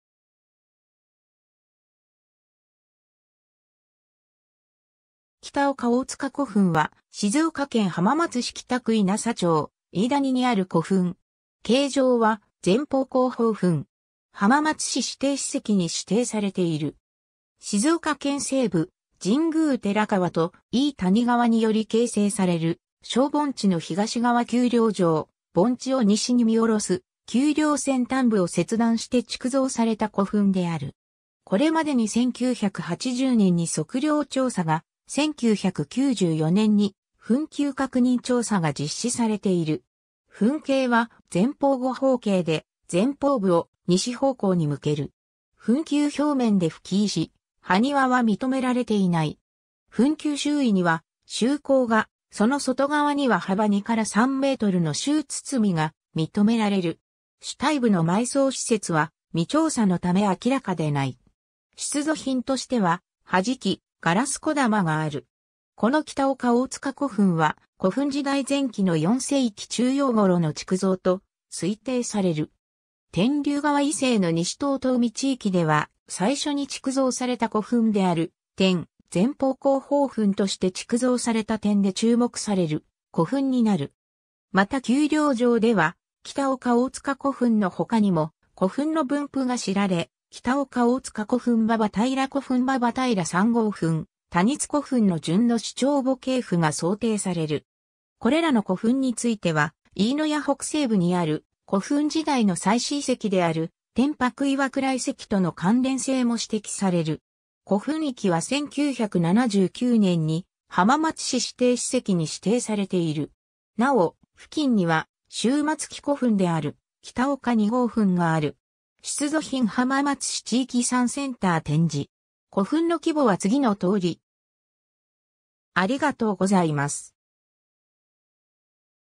北岡大塚古墳
北岡大塚古墳（きたおかおおつかこふん）は、静岡県浜松市北区引佐町井伊谷にある古墳。
形状は前方後方墳。
浜松市指定史跡に指定されている。
 静岡県西部、神宮寺川と井伊谷川により形成される小盆地（井伊谷）の東側丘陵上、盆地を西に見下ろす丘陵先端部を切断して築造された古墳である。
これまでに1980年（昭和55年）に測量調査が、1994年（平成6年）に墳丘確認調査が実施されている。
 墳形は前方後方形で、前方部を西方向に向ける。
墳丘表面で葺石・埴輪は認められていない。
墳丘周囲には周溝が、その外側には幅2-3メートルの周堤が認められる。
主体部の埋葬施設は未調査のため明らかでない。
出土品としては土師器（二重口縁壺）・ガラス小玉がある。
 この北岡大塚古墳は、古墳時代前期の4世紀中葉頃の築造と推定される。
天竜川以西の西遠江地域では最初に築造された古墳である点、前方後方墳として築造された点で注目される古墳になる。
また丘陵上では北岡大塚古墳のほかにも古墳の分布が知られ、北岡大塚古墳→馬場平古墳→馬場平3号墳→谷津古墳の順の首長墓系譜が想定される。
これらの古墳については、井伊谷北西部にある古墳時代の祭祀遺跡である天白磐座遺跡との関連性も指摘される。
 古墳域は1979年（昭和54年）に浜松市指定史跡に指定されている。
なお、付近には終末期古墳である北岡2号墳（浜松市指定史跡）がある。
 出土品浜松市地域遺産センター展示。